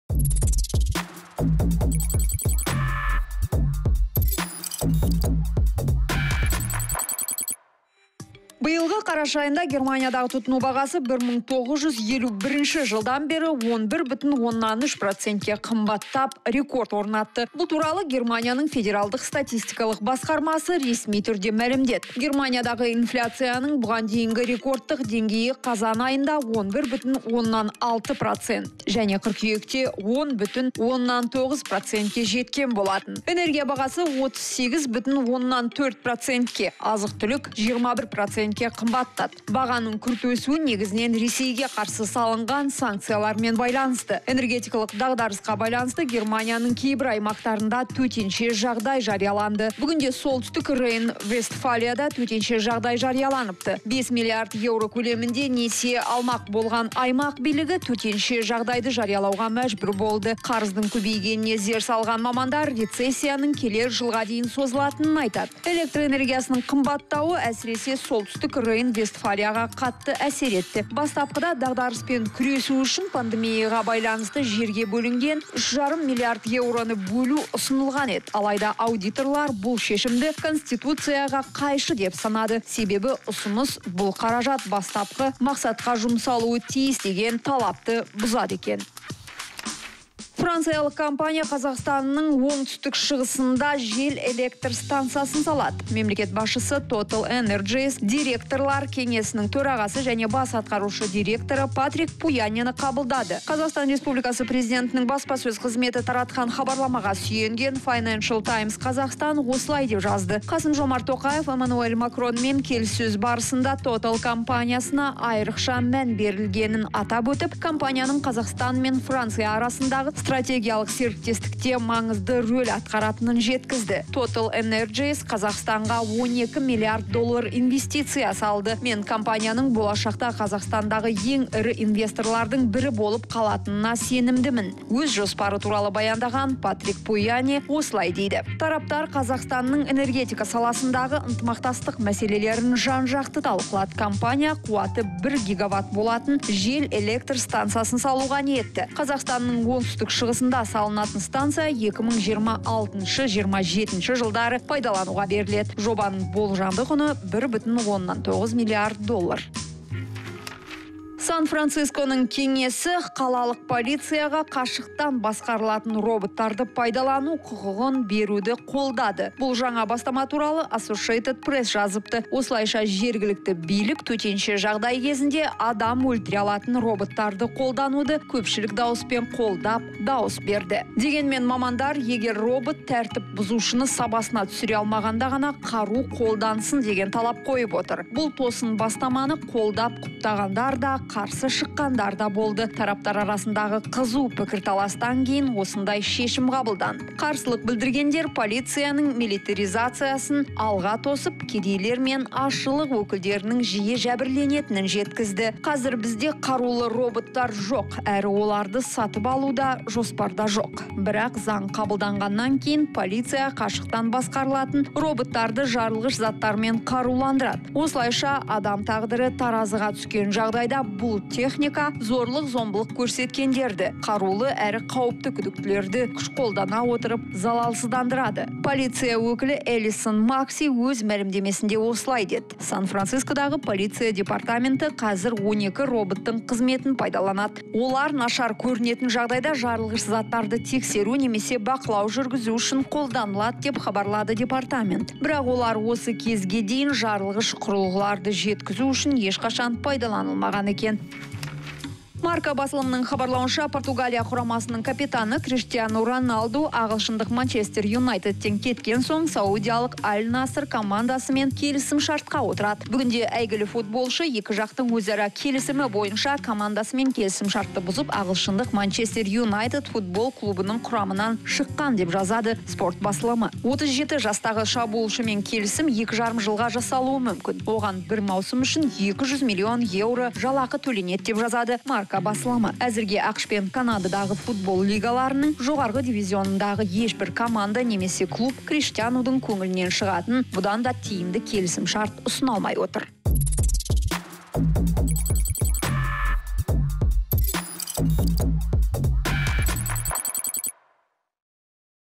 Франциялық компания Қазақстанның оңтүстік-шығысында жел электр стансасын салады | Баспасөз
Бүгінгі бағдарламада:

- Биылғы қараша айында Германиядағы тұтыну бағасы 1951 жылдан бері 11,3% қымбаттап, рекорд орнатты;
- Франциялық компания Қазақстанның оңтүстік-шығысында жел электр стансасын салады;
- Сан-Францисконың кеңесі қалалық полицияға қашықтан басқарылатын роботтарды пайдалану құқығын беруді қолдады;
- Әйгілі футболшы Криштиану Роналду ағылшындық «Манчестер Юнайтедтен» кеткен соң, саудиялық «Аль-Наср» командасымен келісімшартқа отырмақ.